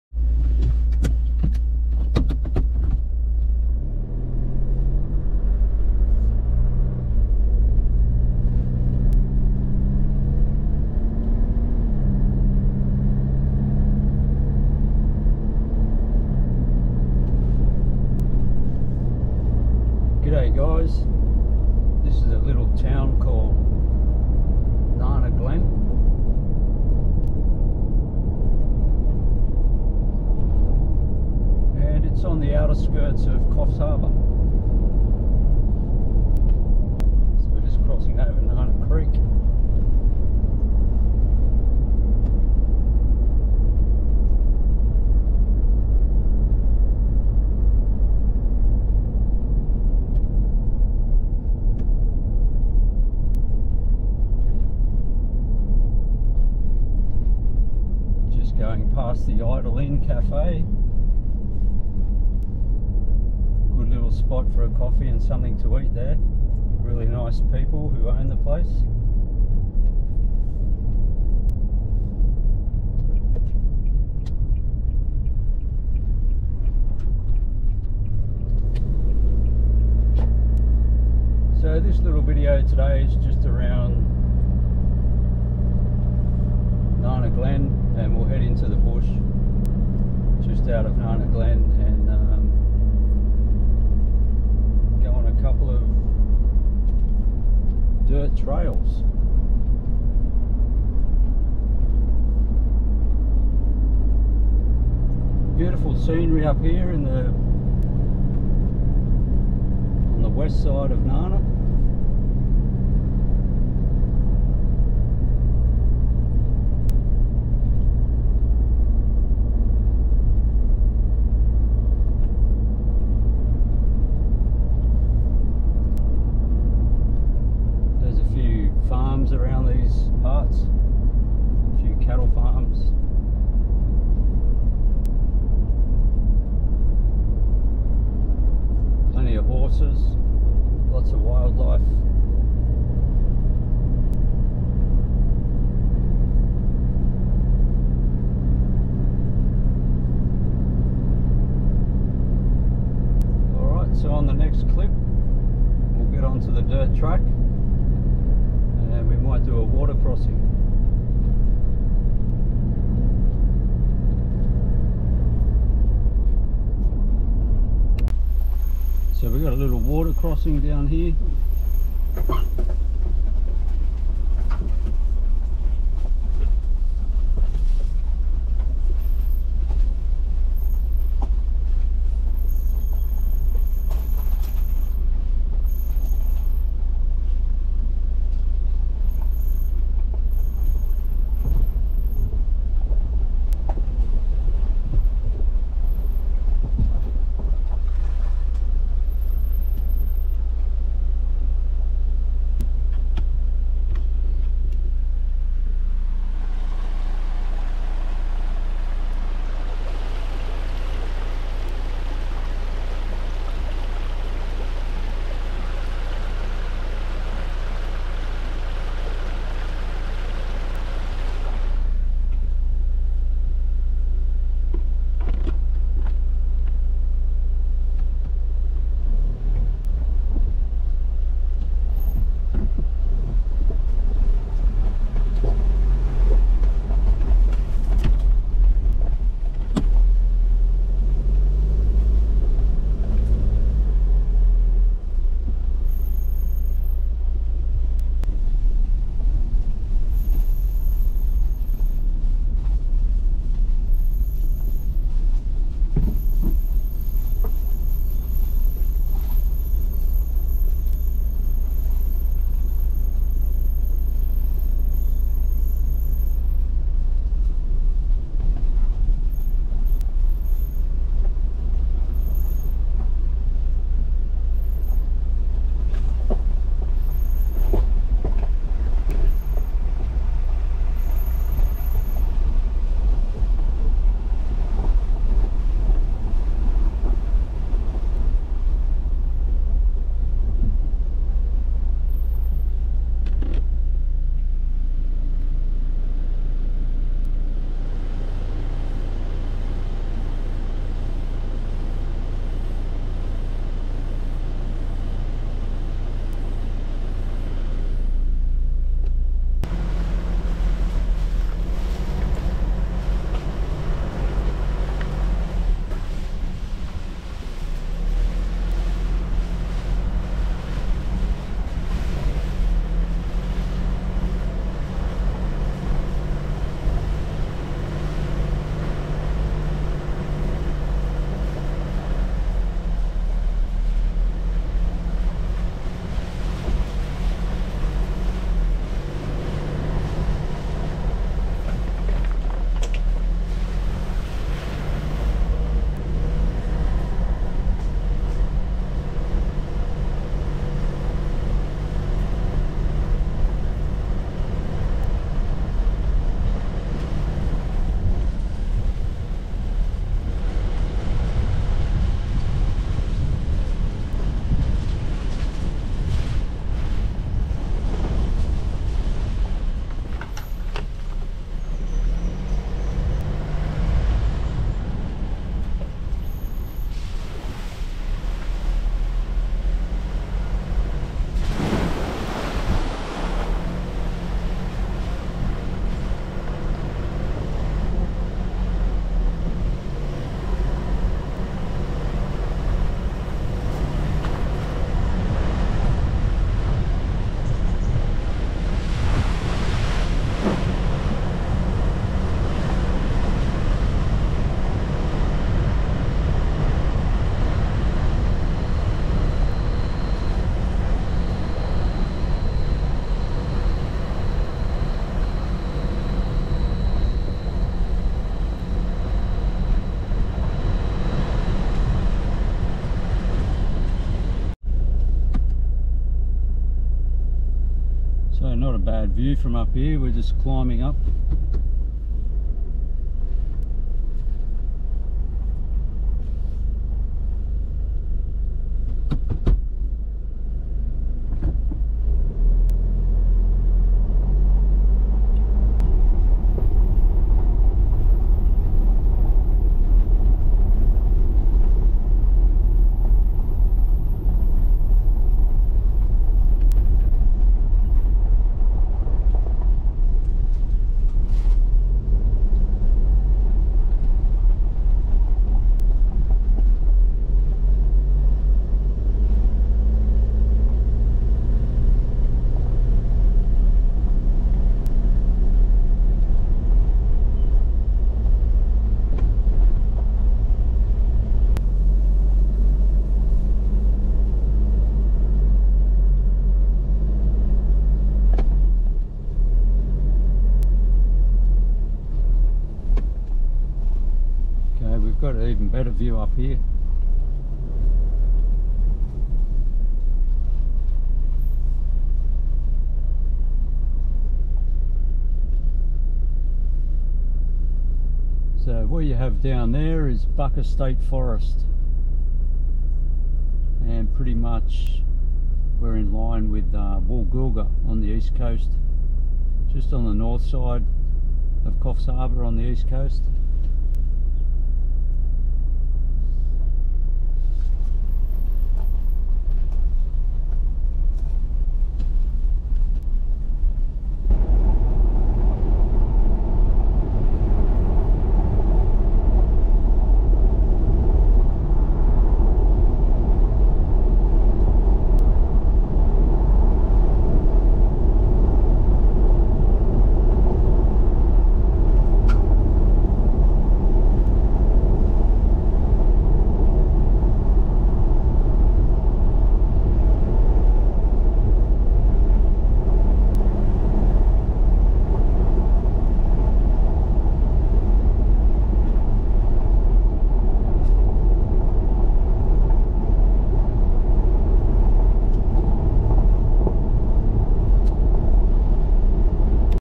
Driving-thru Nana Glen then into the bush over a water crossing
In this clip I drive through Nana Glen Coffs Harbour then turn off towards the Bagawa State Forest and cross over a creek crossing then back again towards Bushman rangers Rd